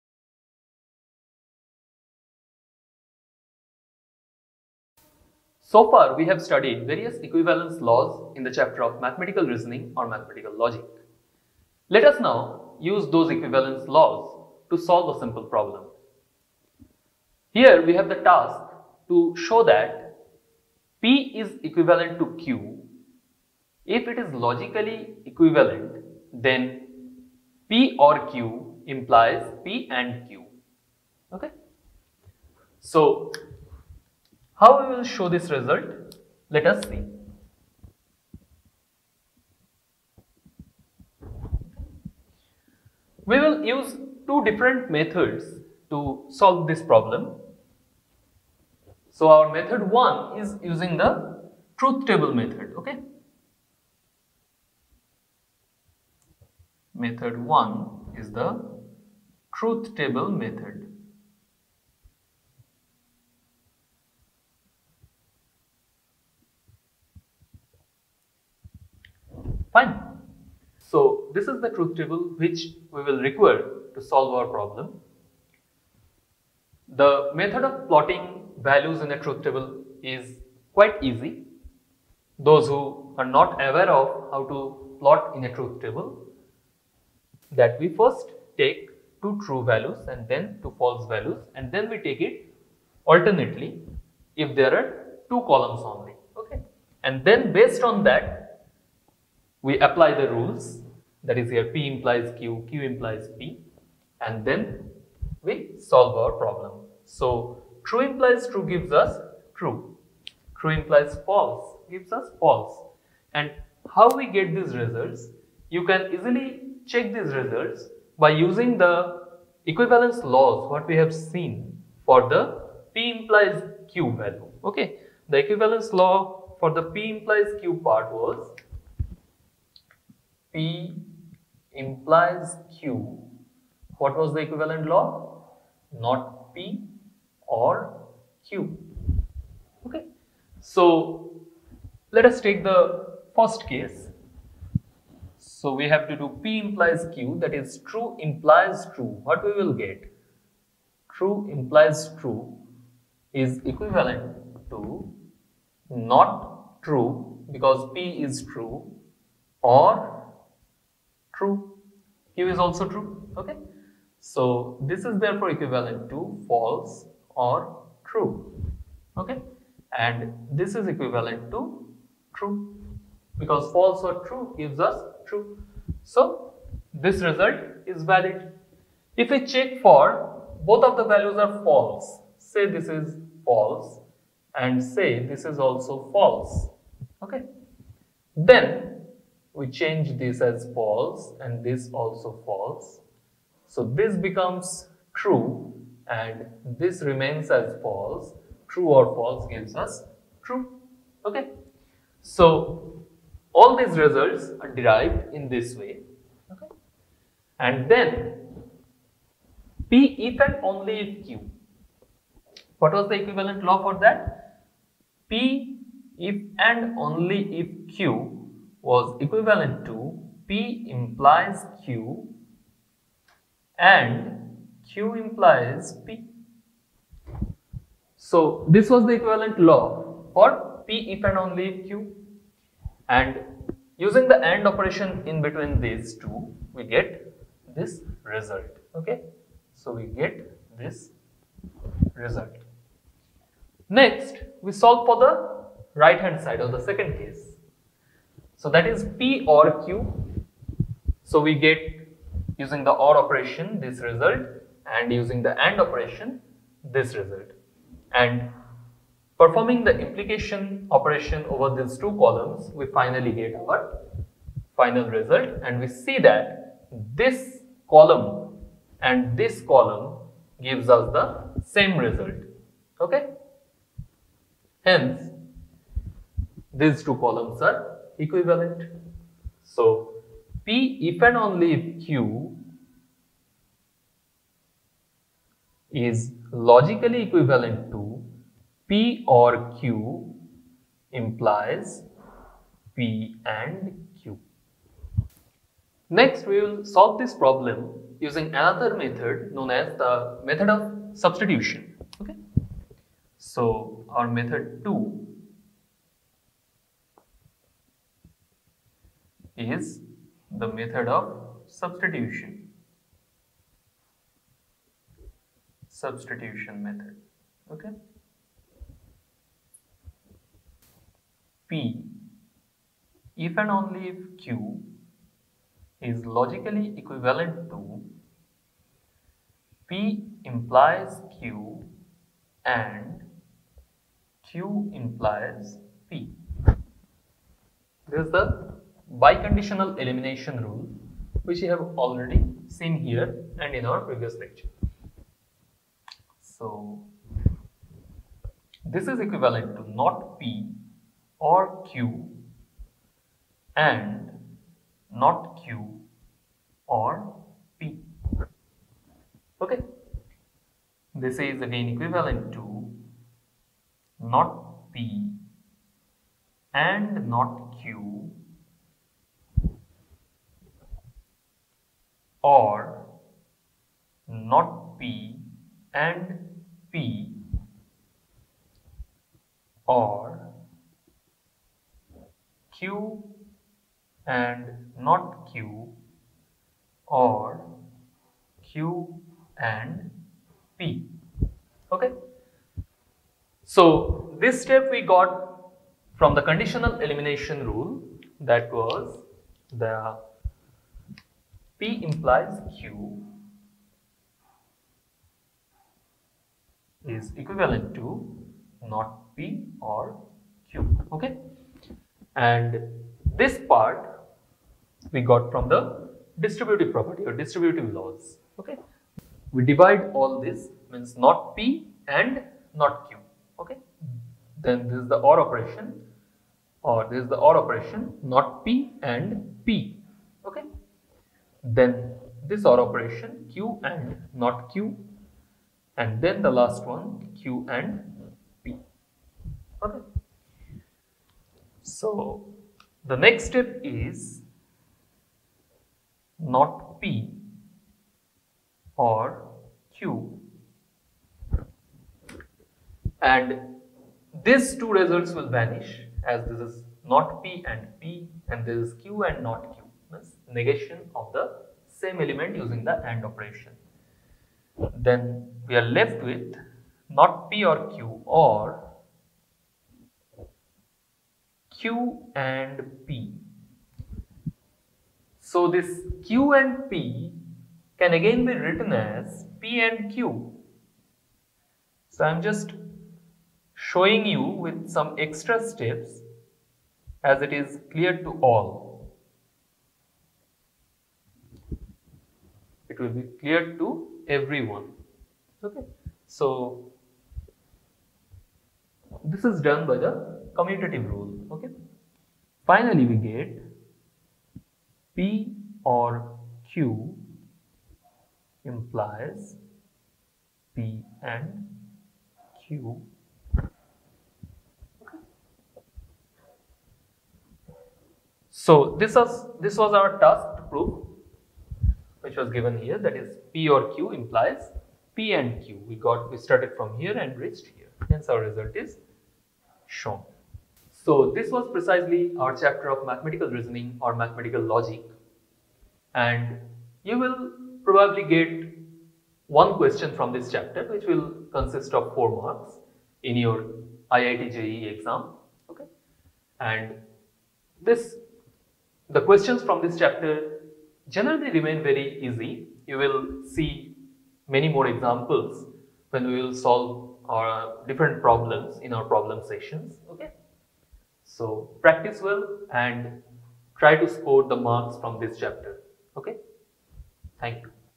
Continued .. Example of Mathematical Logic & Reasoning(Statement and Negation) | CBSE Mathematics XI
Mathematical logic is a sub-field of mathematics exploring the applications of formal logic to mathematics.
For easy understanding, presenting an Example in this video !!!